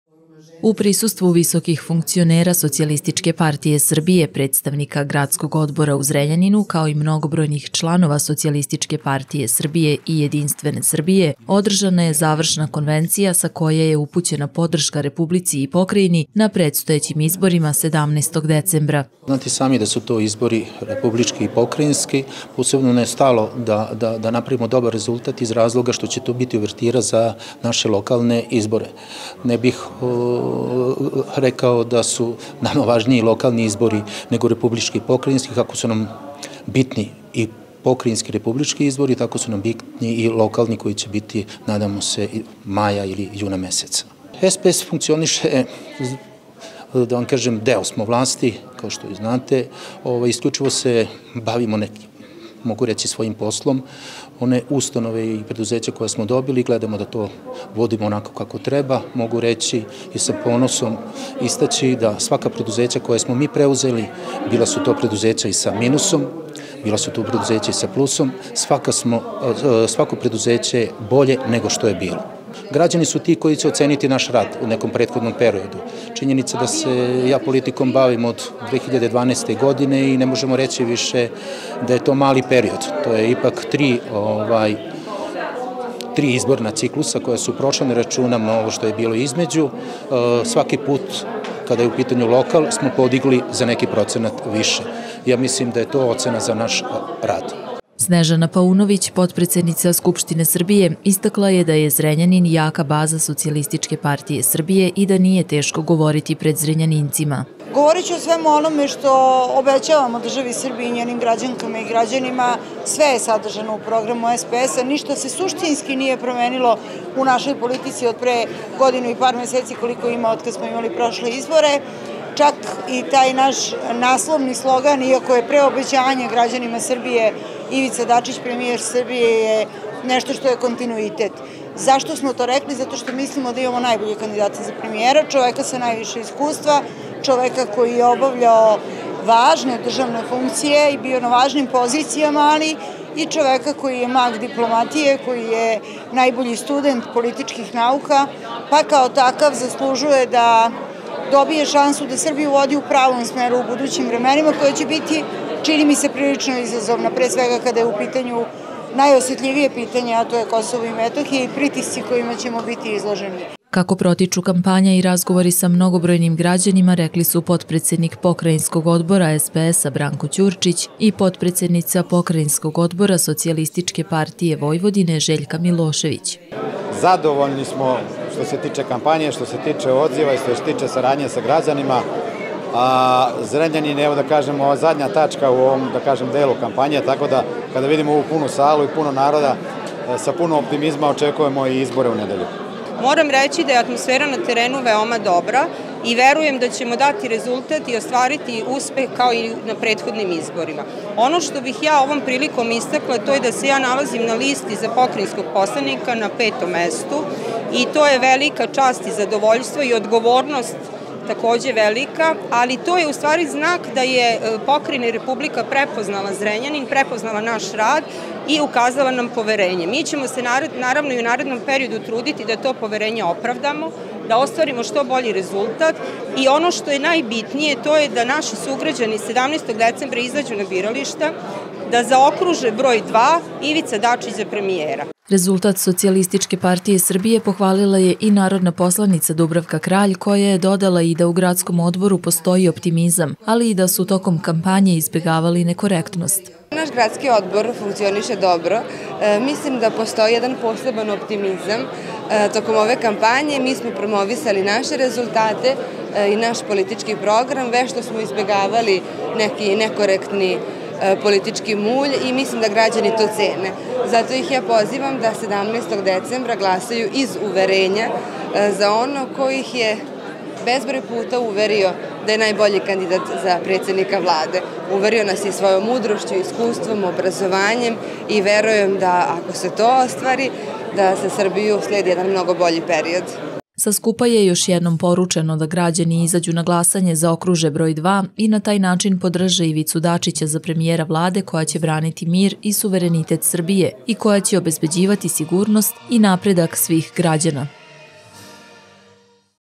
Završna konvencija Gradskog odbora SPS-a u Zrenjaninu
U sali hotela Vojvodina održana je završena konvencija Gradskog odbora Socijalističke partije Srbije u Zrenjaninu čime je data podrška republici i pokrajini.